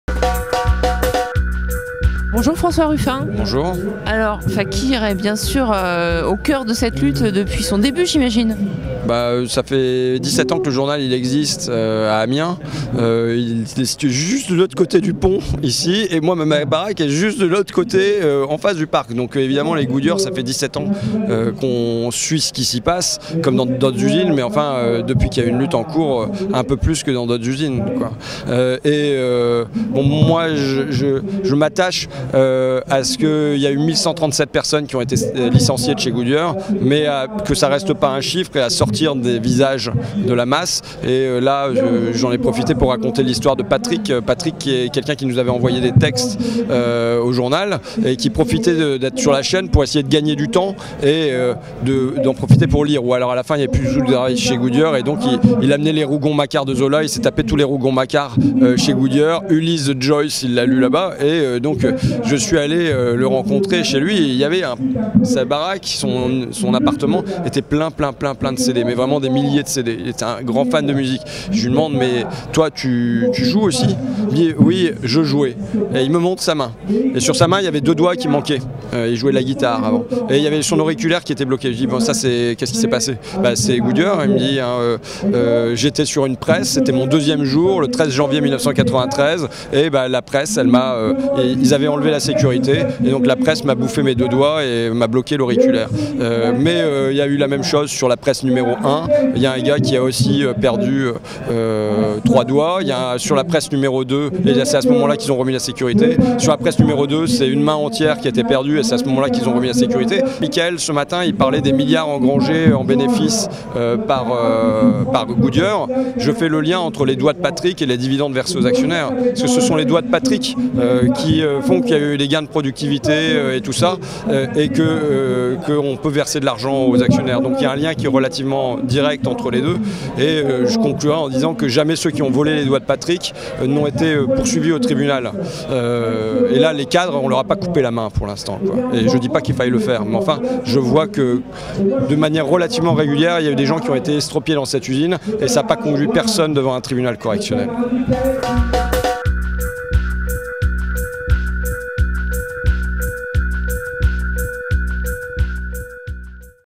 François Ruffin rend hommage à Patrick, ex Goodyear qui y a laissé trois doigts
Venu en voisin, le fondateur de Fakir et réalisateur de "Merci patron" suit les luttes des salariés de Goodyear depuis la création de son journal. Il s'attache aujourd'hui à "sortir des visages" de la masse des licenciés de l'usine désormais fermée. Il nous raconte l'histoire de Patrick, un ex-ouvrier de Goodyear, emblématique d'un système qui broie des hommes pour toujours plus de profits.